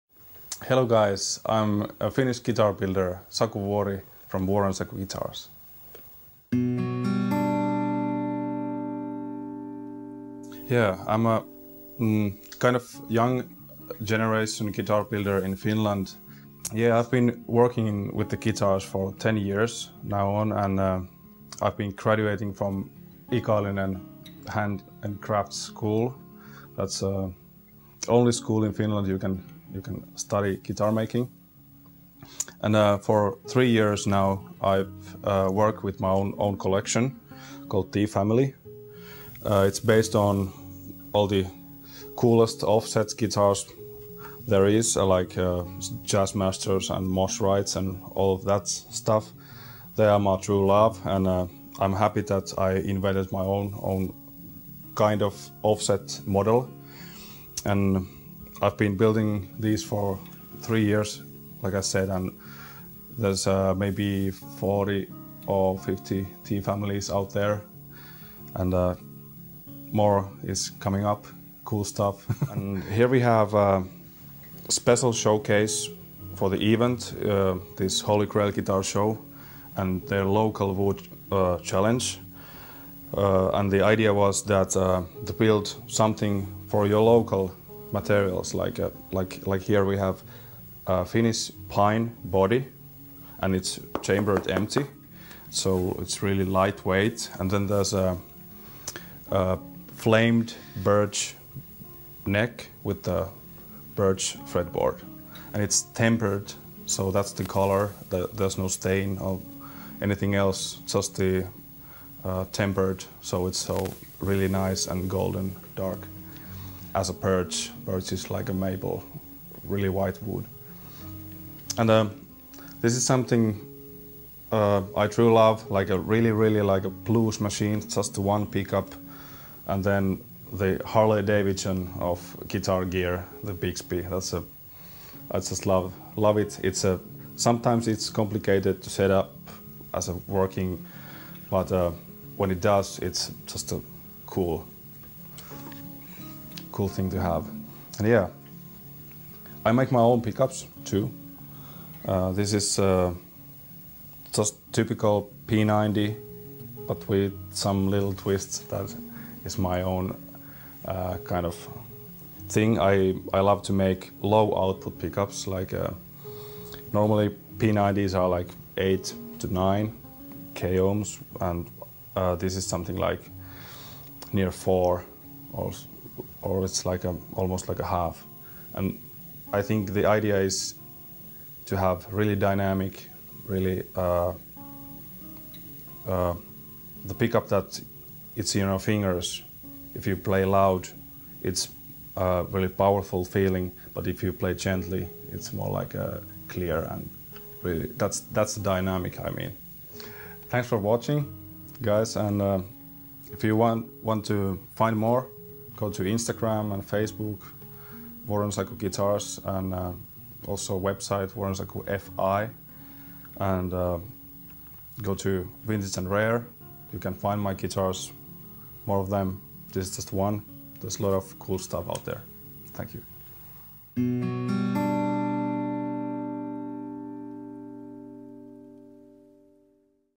VuorenSaku Guitars / Interview / VintageandRare.com / Holy Grail Guitar Show
Interview with Finnish luthier, Saku Vuori, from VuorenSaku Guitars. Done on Holy Grail Guitar Show 2018 exclusively for V&R.